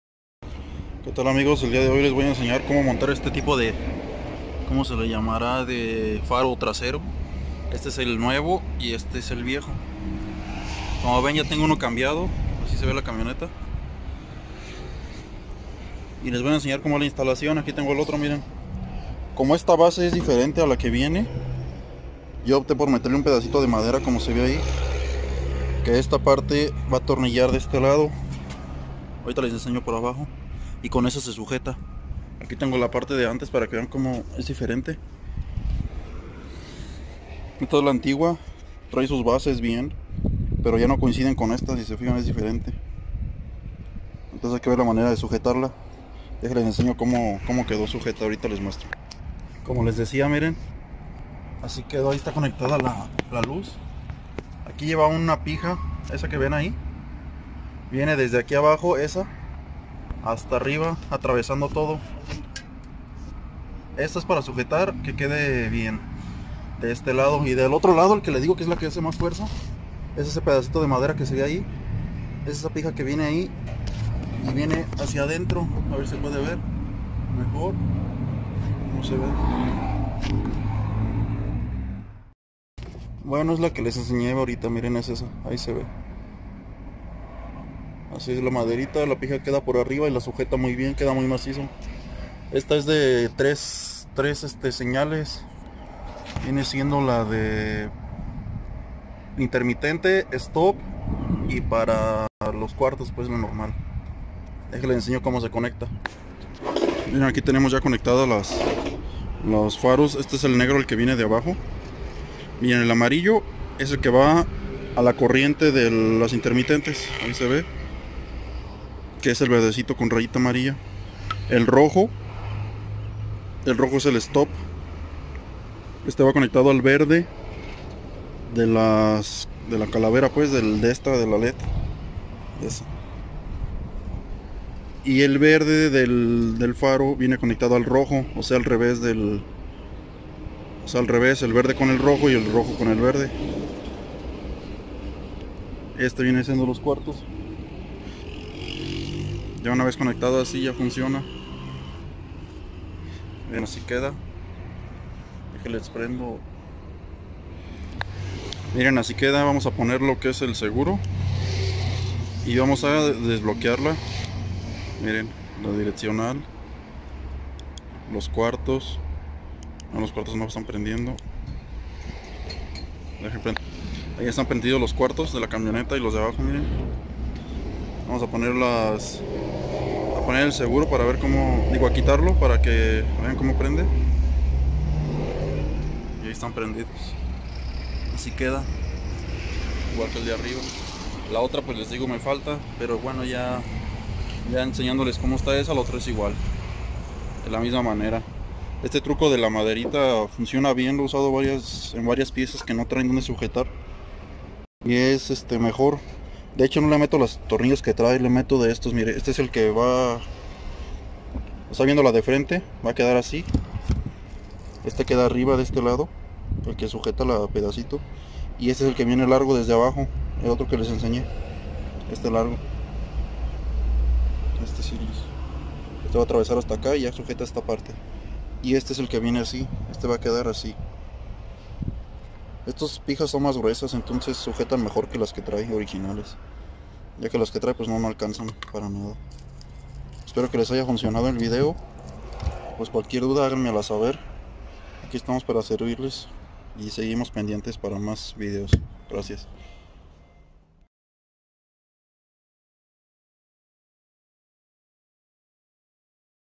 Instalación faro o reflector TRASERO HONDA HR-V
Proceso de instalación de faro o reflector trasero.